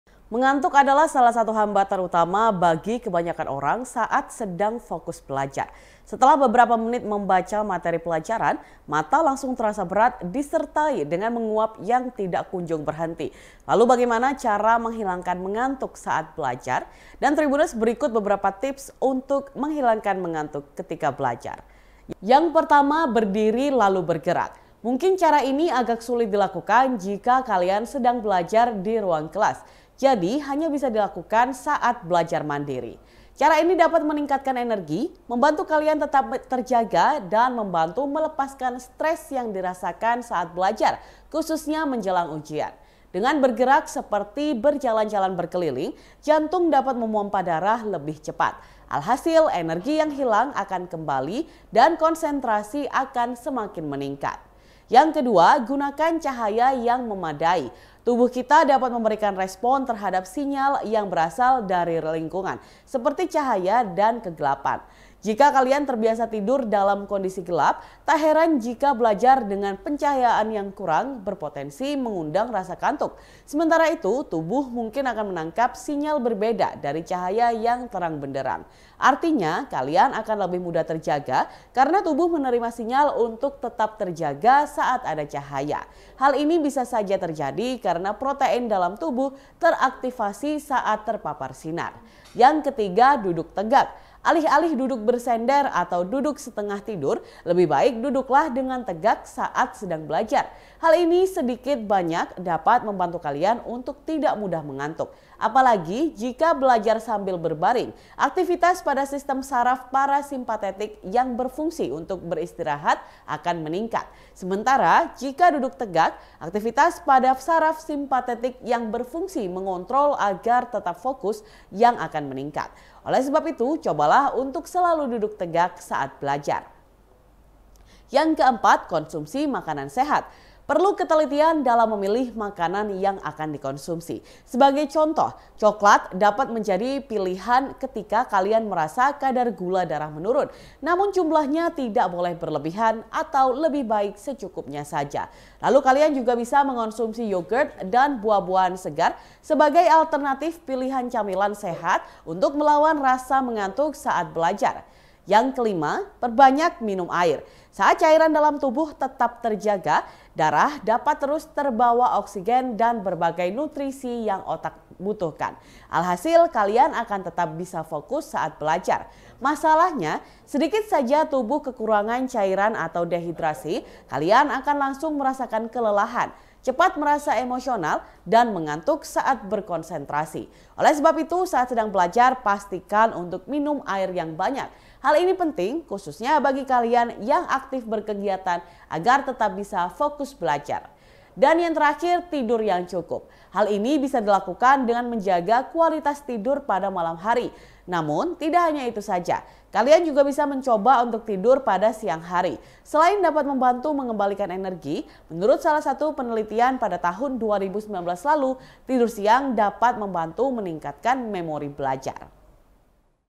Ini Tips Effektif yang Bisa Hilangkan Ngantuk Saat Belajar
TRIBUNJATIMTIMUR.COM - Mengantuk adalah salah satu hambatan utama bagi kebanyakan orang saat sedang fokus belajar.

Setelah beberapa menit membaca materi pelajaran, mata langsung terasa berat disertai dengan menguap yang tak kunjung berhenti.

Lalu, bagaimana cara menghilangkan mengantuk saat belajar?

1. Berdiri lalu bergerak

Mungkin, cara ini agak sulit dilakukan jika kalian sedang belajar di ruang kelas, jadi hanya bisa dilakukan saat belajar mandiri.

Cara ini dapat meningkatkan energi, membantu kalian tetap terjaga, dan membantu melepaskan stres yang dirasakan saat belajar, khususnya menjelang ujian.

Dengan bergerak seperti berjalan-jalan berkeliling, jantung dapat memompa darah lebih cepat.

Alhasil, energi yang hilang akan kembali dan konsentrasi akan semakin meningkat.

2. Gunakan cahaya yang memadai

Tubuh kita dapat memberikan respons terhadap sinyal yang berasal dari lingkungan, seperti cahaya dan kegelapan.

Jika kalian terbiasa tidur dalam kondisi gelap, tak heran jika belajar dengan pencahayaan yang kurang berpotensi mengundang rasa kantuk.

Sementara itu, tubuh mungkin akan menangkap sinyal berbeda dari cahaya yang terang benderang.

Artinya, kalian akan lebih mudah terjaga karena tubuh menerima sinyal untuk tetap terjaga saat ada cahaya.

Hal ini bisa saja terjadi karena protein dalam tubuh teraktivasi saat terpapar sinar.

3. Duduk tegak

Alih-alih duduk bersender atau duduk setengah tidur, lebih baik duduklah dengan tegak saat sedang belajar.

Hal ini sedikit banyak dapat membantu kalian untuk tidak mudah mengantuk.

Apalagi, jika belajar sambil berbaring, aktivitas pada sistem saraf parasimpatetik yang berfungsi untuk beristirahat akan meningkat.

Sementara, jika duduk tegak, aktivitas pada saraf simpatetik yang berfungsi mengontrol agar tetap fokus lah yang akan meningkat.

Oleh sebab itu, cobalah untuk selalu duduk tegak saat belajar.

4. Konsumsi makanan sehat

Perlu ketelitian dalam memilih makanan yang akan dikonsumsi.

Sebagai contoh, cokelat dapat menjadi pilihan ketika kalian merasa kadar gula darah menurun.

Namun, jumlahnya tidak boleh berlebihan, atau lebih baik secukupnya saja.

Lalu, kalian juga bisa mengonsumsi yoghurt dan buah-buahan segar sebagai alternatif pilihan camilan sehat untuk melawan rasa mengantuk saat belajar.

5. Perbanyak minum air

Saat cairan dalam tubuh tetap terjaga, darah dapat terus membawa oksigen dan berbagai nutrisi yang otak butuhkan.

Alhasil, kalian akan tetap bisa fokus saat belajar.

Masalahnya, sedikit saja tubuh kekurangan cairan atau dehidrasi, kalian akan langsung merasakan kelelahan, cepat merasa emosional, dan mengantuk saat berkonsentrasi.

Oleh sebab itu, saat sedang belajar, pastikan untuk minum air yang banyak.

Hal ini penting, khususnya bagi kalianyang aktif berkegiatan agar tetap bisa fokus belajar.

6. Tidur yang cukup

Hal ini bisa dilakukan dengan menjaga kualitas tidur pada malam hari.

Namun, tak hanya itu saja, kalian juga bisa mencoba untuk tidur pada siang hari.

Selain dapat membantu mengembalikan energi, menurut salah satu peneltian tahun 2019, tidur siang juga dapat membantu meningkatkan memori belajar.